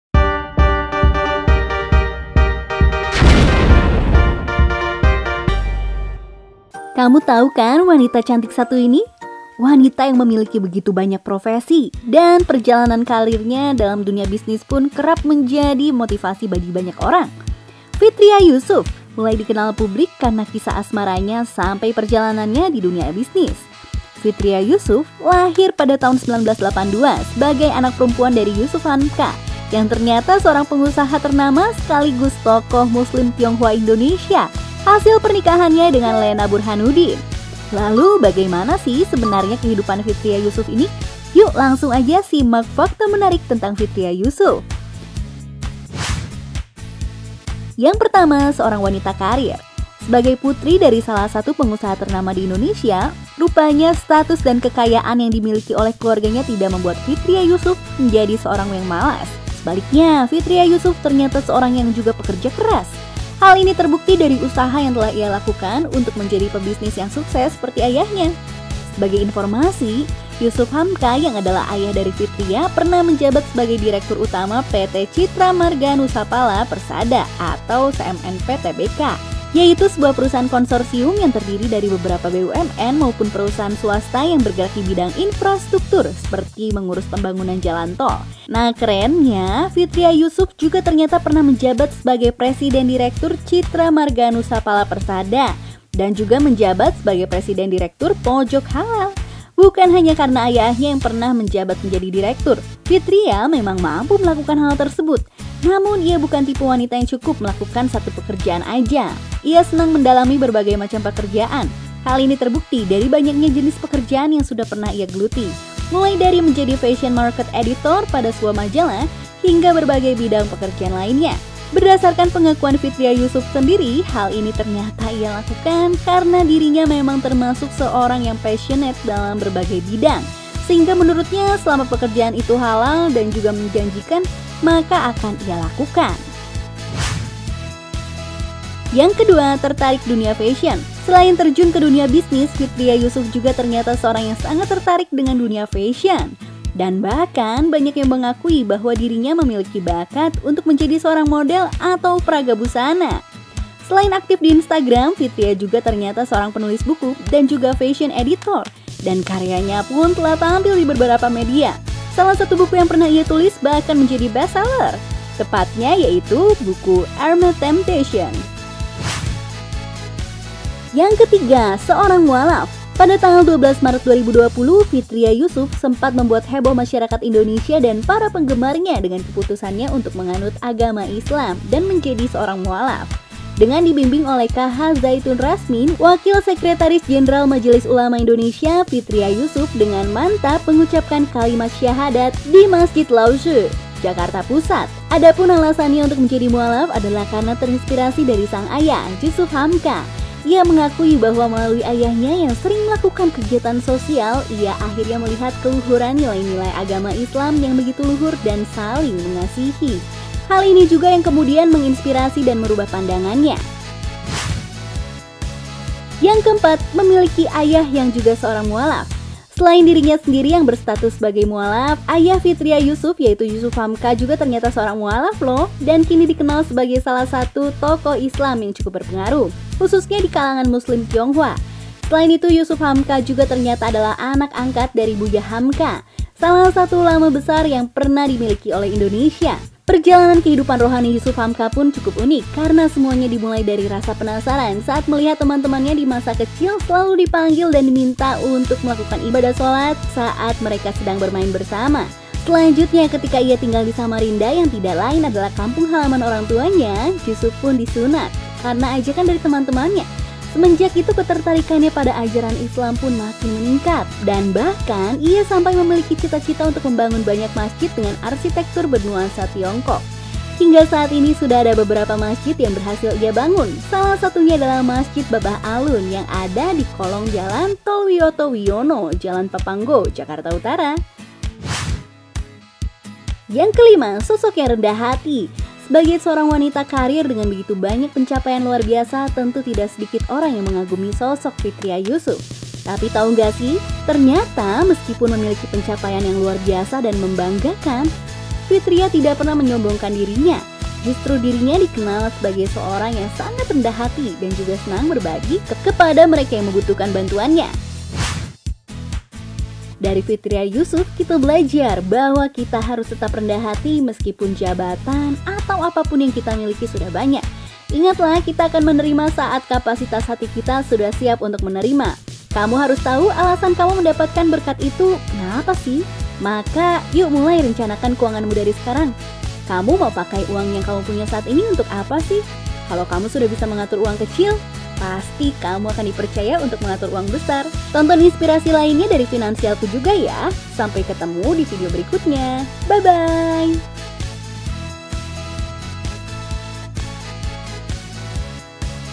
Fitria Yusuf: Direktur Utama Jalan Tol Sekaligus Model yang Jadi Mualaf
Kamu tau wanita cantik satu ini? Wanita yang memiliki begitu banyak profesi, dan perjalanan karirnya dalam dunia bisnis pun kerap menjadi motivasi bagi banyak orang. Fitri Yusuf mulai dikenal publik karena kisah asmaranya hingga perjalanannya di dunia bisnis.
Fitria Yusuf lahir pada tahun 1982 sebagai anak perempuan dari Yusuf Hamka yang ternyata seorang pengusaha ternama sekaligus tokoh Muslim Tionghoa Indonesia, hasil pernikahannya dengan Lena Burhanudin.
Lalu bagaimana sebenarnya kehidupan Fitria Yusuf ini? Yuk, simak beberapa fakta menarik berikut ini.
1. Seorang wanita karir
Sebagai putri dari salah satu pengusaha ternama di Indonesia, rupanya status dan kekayaan yang dimiliki oleh keluarganya tidak membuat Fitria Yusuf menjadi seorang yang malas ataupun menjadi anak yang hanya bisa menikmati harta orang tuanya saja, lho. Sebaliknya, Fitria Yusuf ternyata seorang yang juga pekerja keras! Hal ini terbukti dari usaha yang telah ia lakukan untuk menjadi pebisnis yang sukses seperti ayahnya.
Sebagai informasi, Jusuf Hamka yang adalah ayah dari Fitria pernah menjabat sebagai Direktur Utama PT Citra Marga Nusaplaha Persada (CMNP) Tbk, yaitu sebuah perusahaan konsorsium yang terdiri dari beberapa BUMN maupun perusahaan swasta yang bergerak di bidang infrastruktur, seperti mengurus pembangunan jalan tol. Nah, kerennya Fitria Yusuf juga ternyata pernah menjabat sebagai Presiden Direktur Citra Marga Nusaphala Persada, dan juga menjabat sebagai Presiden Direktur Podjok Halal. Bukan hanya karena ayahnya yang pernah menjabat menjadi direktur, Fitria memang mampu melakukan hal tersebut.
Namun, ia bukan tipe wanita yang cukup melakukan satu pekerjaan saja, ia senang mendalami berbagai macam pekerjaan. Hal ini terbukti dari banyaknya jenis pekerjaan yang sudah pernah ia geluti, mulai dari menjadi Fashion Market Editor pada sebuah majalah hingga berbagai bidang pekerjaan lainnya.
Berdasarkan pengakuan Fitri Yusuf sendiri, hal ini ternyata ia lakukan karena dirinya memang termasuk seorang yang passionate dalam berbagai bidang, sehingga menurutnya selama pekerjaan itu halal dan juga menjanjikan maka akan ia lakukan.
2. Tertarik Dunia Fashion
Selain terjun ke dunia bisnis, Fitri Yusuf juga ternyata seorang yang sangat tertarik dengan dunia fashion lho, dan bahkan banyak yang mengakui bahwa dirinya memiliki bakat untuk menjadi seorang model atau peraga busana. Selain aktif di Instagram, Fitria juga ternyata seorang penulis buku dan juga fashion editor, dan karyanya pun telah tampil di beberapa media. Salah satu buku yang pernah ia tulis bahkan menjadi best seller lho, tepatnya yaitu buku Hermes Temptation
3. Seorang Mualaf
Pada tanggal 12 Maret 2020, Fitria Yusuf sempat membuat heboh masyarakat Indonesia dan para penggemarnya dengan keputusannya untuk menganut Agama Islam dan menjadi mualaf. Dengan dibimbing oleh KH Zaitun Rasmin, Wakil Sekretaris Jenderal Majelis Ulama Indonesia, Fitria Yusuf dengan mantap mengucapkan kalimat syahadat di Masjid Lautze, Jakarta Pusat.
4. Memiliki ayah yang juga seolah mualaf
Selain dirinya sendiri yang berstatus sebagai mualaf, ayah Fitri Yusuf, yaitu Jusuf Hamka juga ternyata seorang mualaf lho, dan kini dikenal sebagai salah satu tokoh Islam yang cukup berpengaruh, khususnya dikalangan muslim Tionghoa. Selain itu Jusuf Hamka juga ternyata adalah anak angkat dari Buya Hamka, salah satu ulama besar yang pernah dimiliki oleh Indonesia.